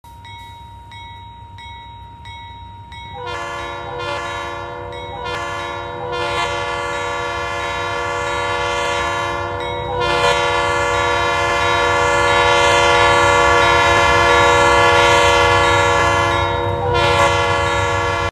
Railfanning CSX Q189 Power
This video was taken from A&G Model Railclub’s Green Ash division.